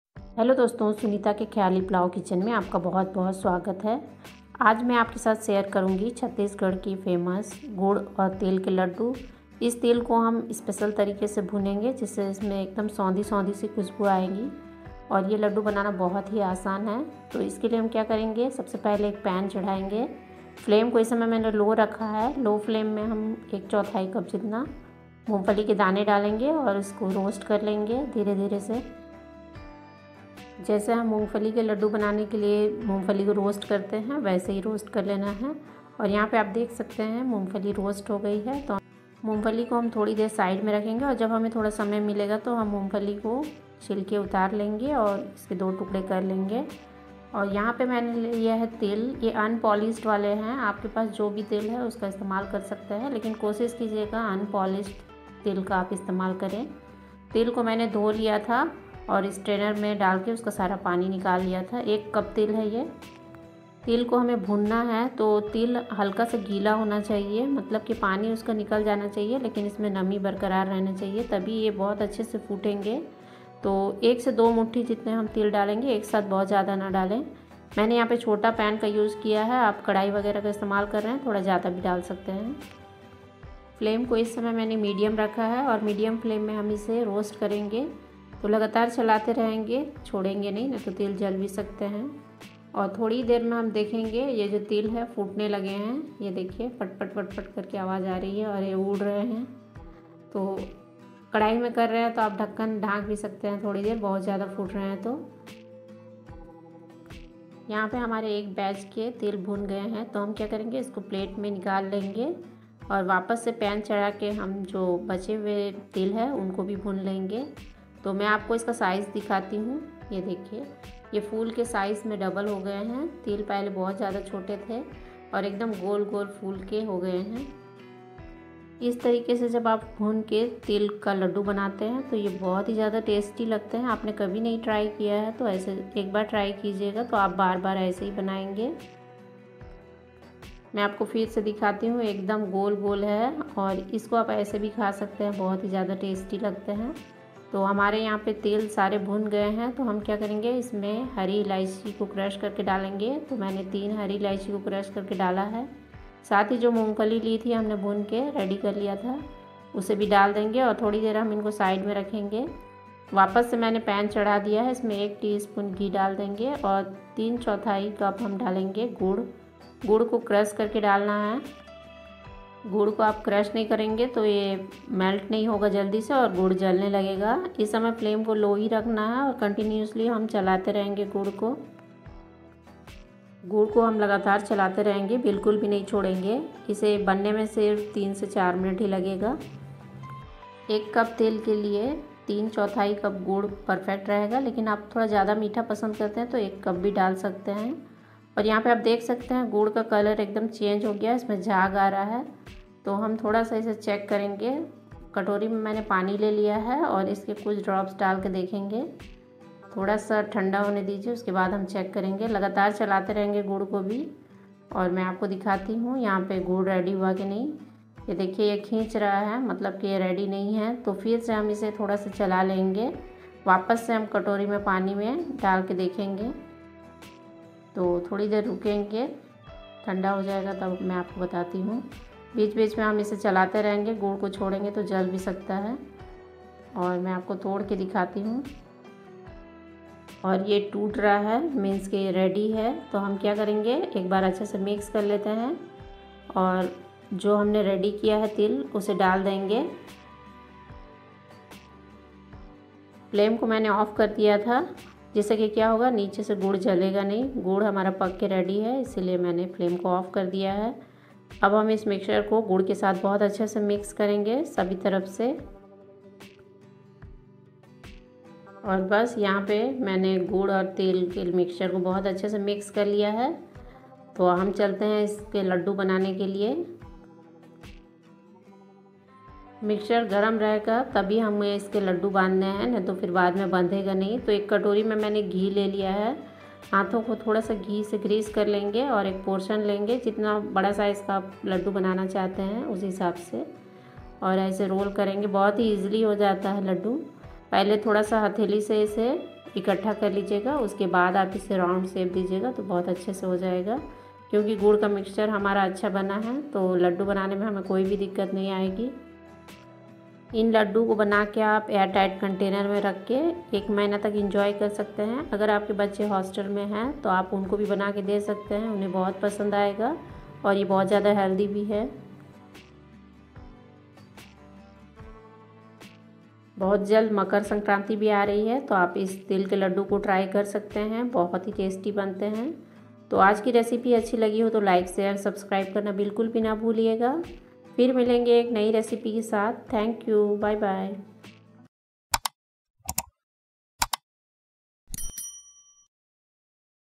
तिल गुड़ के लड्डू बनाने का देसी तरीका | छत्तीसगढ़ के स्पेशल तिल गुड़ लड्डू
छत्तीसगढ़ के स्पेशल तिल गुड़ लड्डू | तिल गुड़ के स्वादिष्ट लड्डू | मकर संक्रांति स्पेशल रेसिपी
How to make til gud laddo | Roasted til laddoo | Jaggery laddo |

Ingredients :
Seasame [ til ] seeds - 1 cup
Peanut - 1/4 cup
Jaggery [ gud ] - 3/4 cup